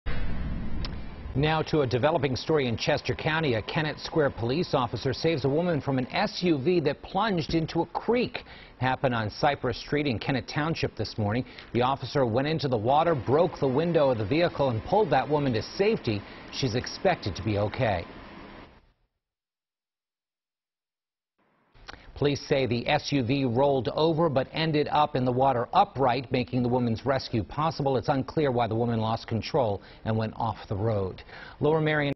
Officer Saves Woman From SUV After It Plunges Into Creek In Kennett Square
The officer went into the water, broke the window, and pulled the woman to safety.